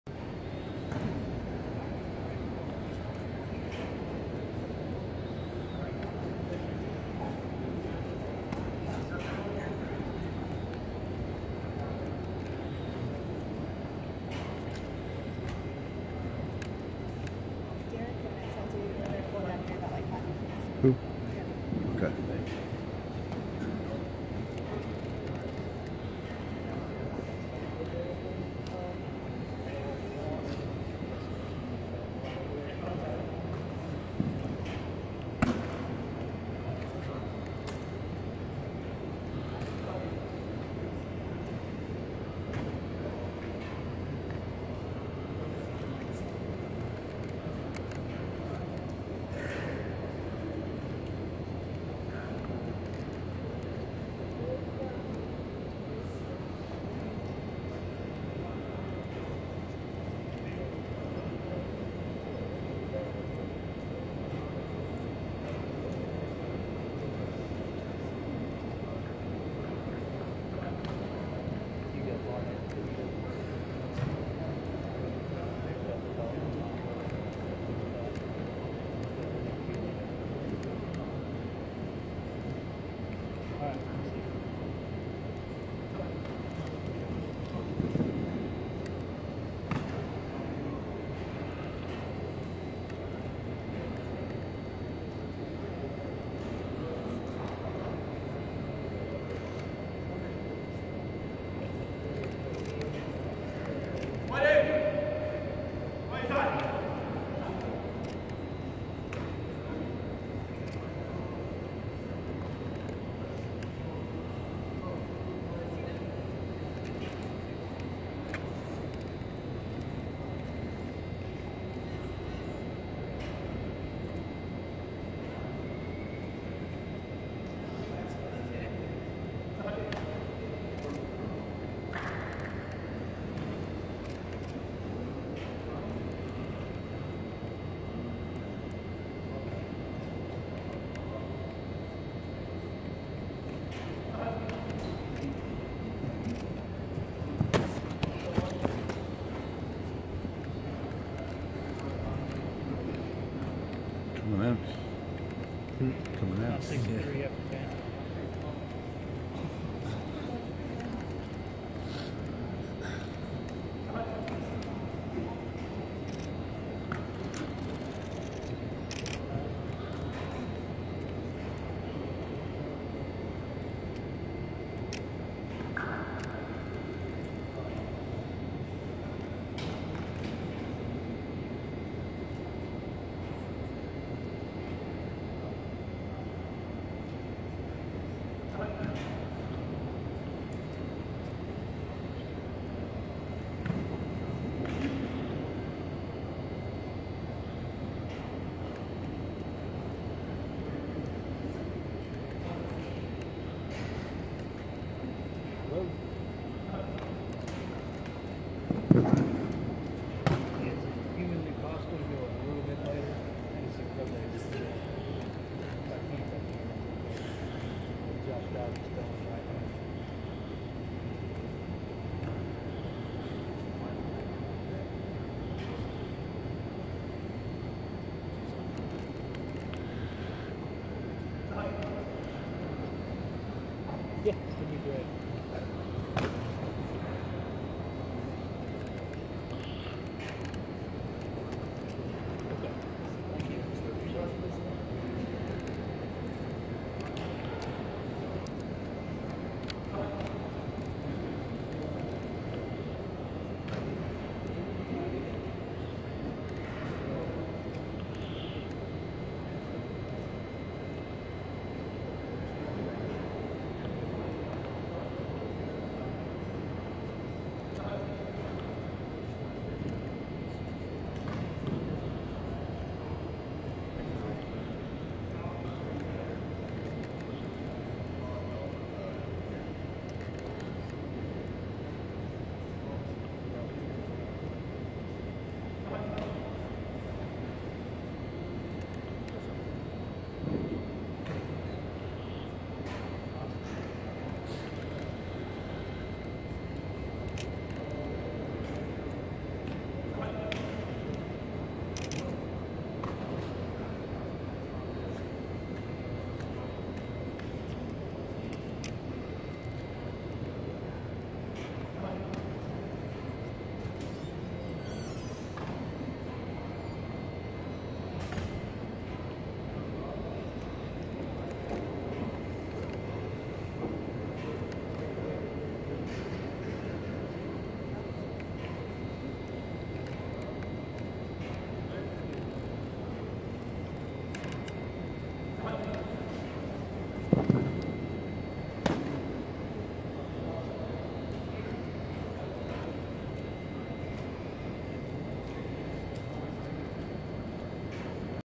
U of Tennessee QB Joshua Dobbs at Pro Day
Throwing to WRs, TEs & RBs. March 31, 2017. Knoxville.